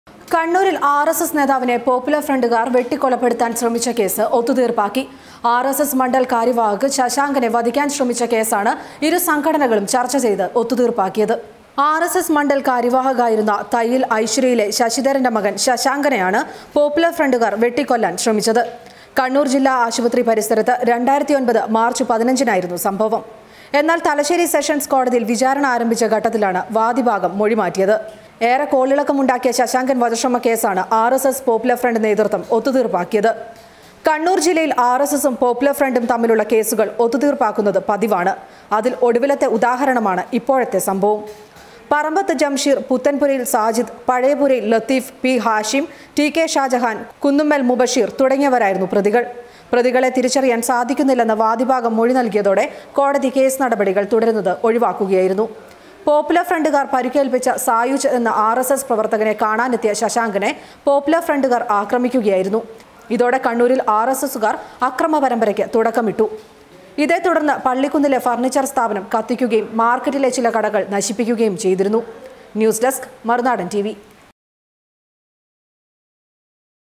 ബദ്ധവൈരികൾ വധശ്രമക്കേസ് ഒത്തുതീർപ്പാക്കിയ കഥ | rss murder case
കണ്ണൂരില്‍ ആര്‍ എസ് എസ് നേതാവിനെ പോപ്പുലര്‍ ഫ്രണ്ടുകാര്‍ വെട്ടിക്കൊല്ലാന്‍ ശ്രമിച്ച കേസ് ഒത്തുതീര്‍പ്പാക്കി. ആര്‍ എസ് എസ് മണ്ഡല്‍ കാര്യവാഹക് ശശാങ്കനെ വധിക്കാന്‍ ശ്രമിച്ച കേസാണ് ഇരു സംഘടനകളും ചര്‍ച്ച ചെയ്ത് ഒത്തുതീര്‍പ്പാക്കിയത്.